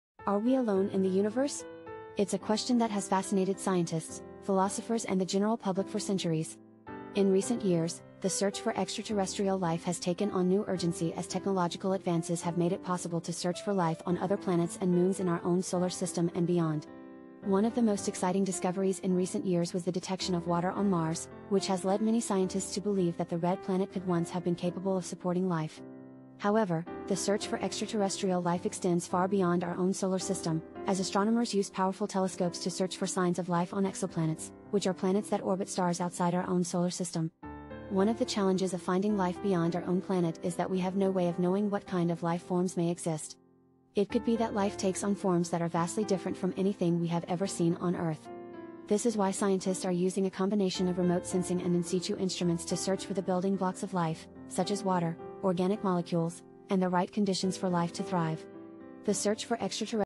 Are we alone in the universe ? The Search for Extraterrestrial Life Takes a Giant Leap!
Explore the ongoing search for extraterrestrial life and the possibilities of alien civilizations existing in our universe. From recent scientific studies to historical evidence, dive into the world of extraterrestrial life and the quest to discover the truth about its existence.
The search for extraterrestrial life has captivated the human imagination for centuries. With the vastness of the universe and the increasing number of exoplanets being discovered, the possibility of finding life beyond our own planet has become a tantalizing and real possibility. The search for extraterrestrial life involves a range of scientific disciplines, including astronomy, biology, and astrobiology.

One of the main questions that scientists are trying to answer is whether we are alone in the universe or if there are other civilizations out there. To answer this question, scientists are using a variety of techniques and instruments, including radio telescopes, to search for signs of life in the form of radio signals or other evidence.

In recent years, the search for extraterrestrial life has taken on new urgency, with several missions being launched to explore the possibility of life on other planets and moons in our solar system. For example, the Mars Rover mission has been exploring the Red Planet in search of signs of past or present life, while the Kepler space telescope has been hunting for exoplanets that are in the habitable zone, where conditions are suitable for life.

The search for extraterrestrial life raises many questions and implications for our understanding of the universe and our place in it. If we do find evidence of life beyond our own planet, it could have a profound impact on our society, our beliefs, and our understanding of the universe.

In conclusion, the search for extraterrestrial life is a complex and exciting field that continues to captivate scientists and the public alike. Despite the many challenges and obstacles, the quest to answer the question "Are we alone in the universe?" continues, and the possibility of finding life beyond our own planet remains a tantalizing and real possibility.
Are we alone in the universe